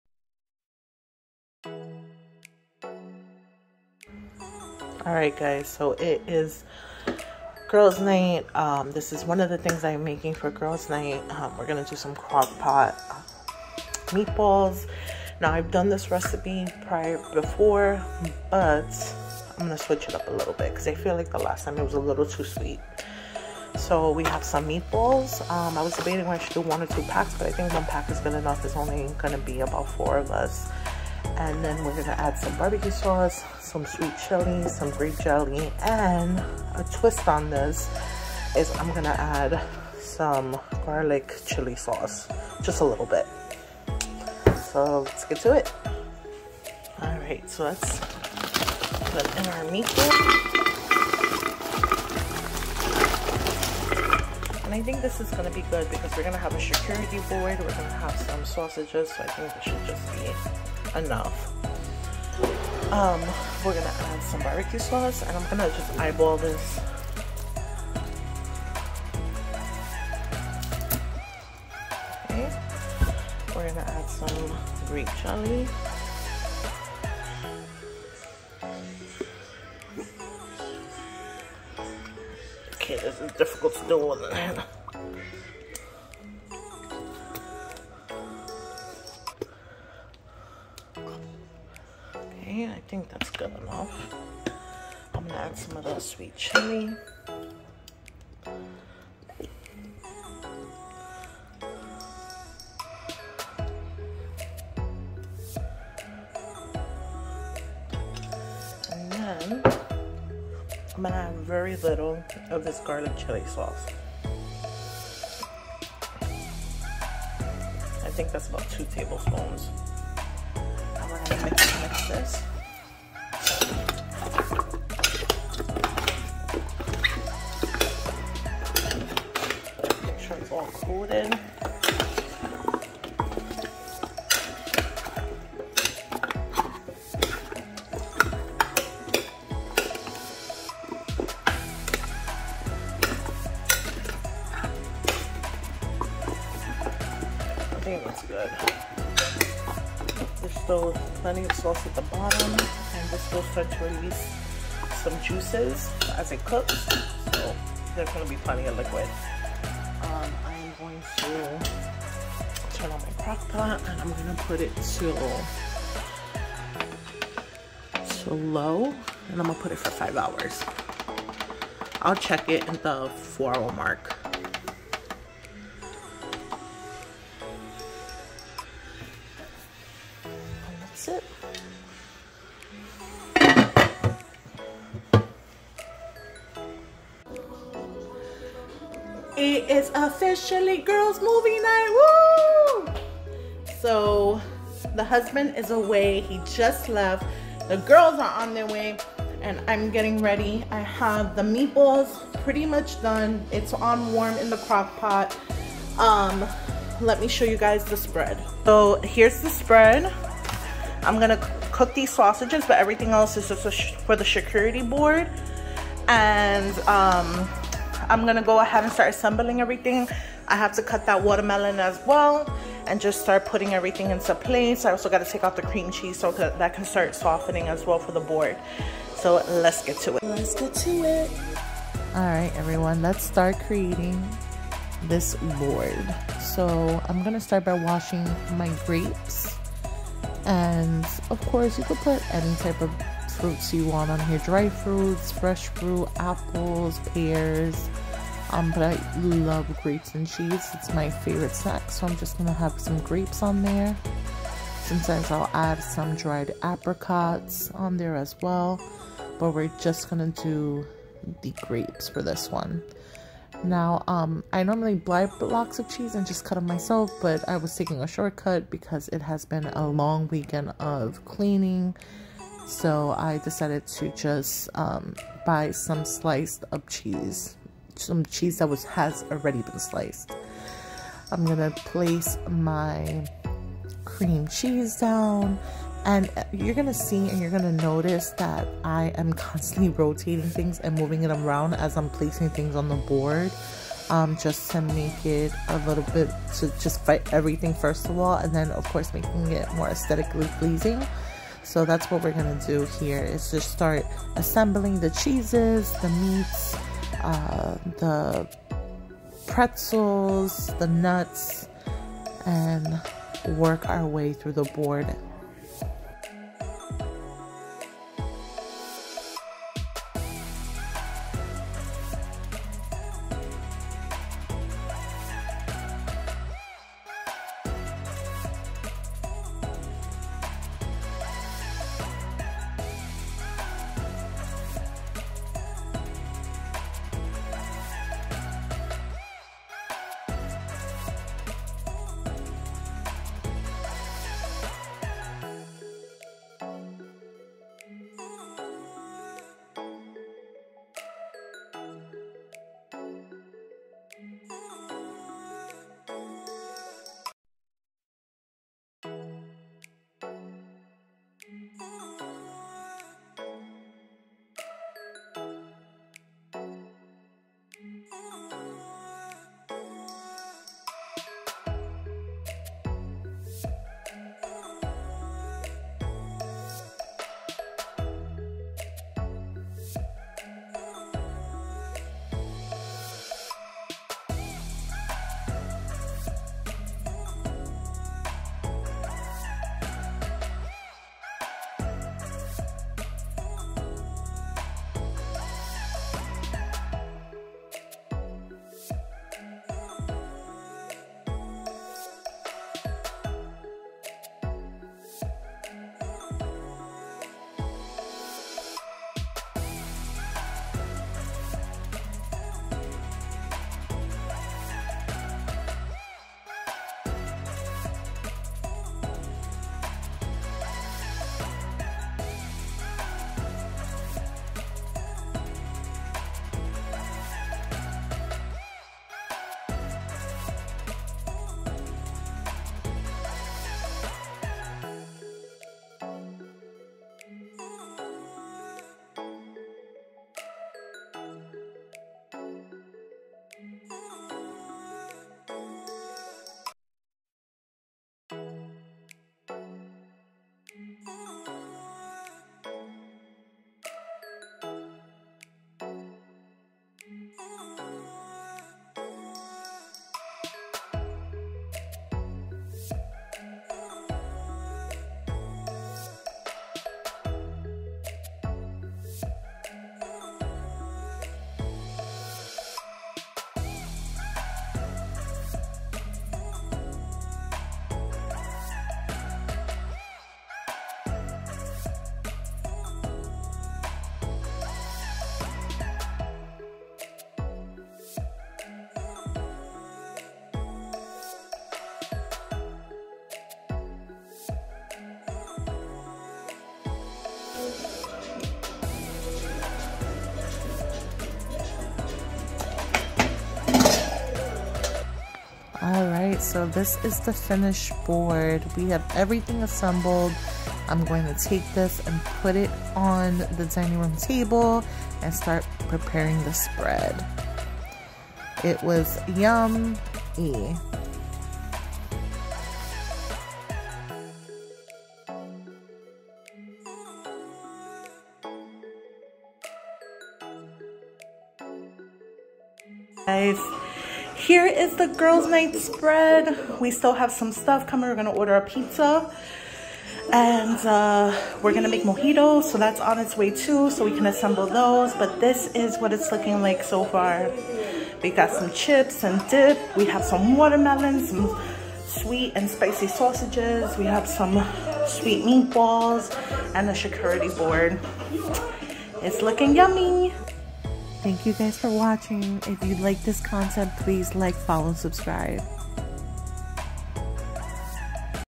Let's get ready for Girls Night!
come along as I prepare for girls movie night. girlsnight girlsnightin charcuterie charcuterieboard appetizers movienight movienight